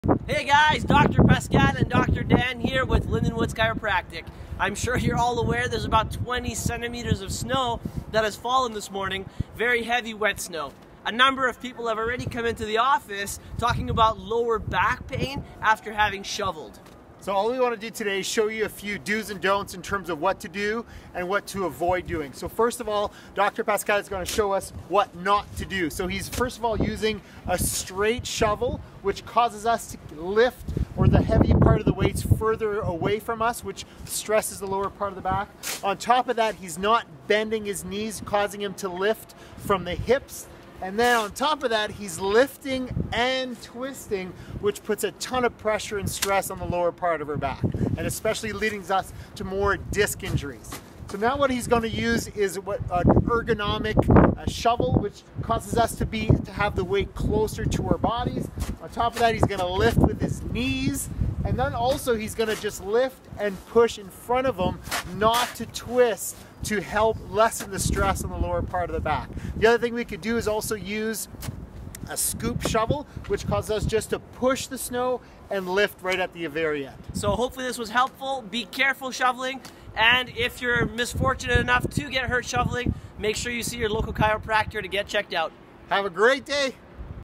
How to shovel snow properly and avoid lower back pain
With all this snow we just got, Winnipeg Chiropractors will show you what is the best way to shovel so you can protect your back! Here is a quick 90sec video on the best and worst things you can do for your back while shoveling.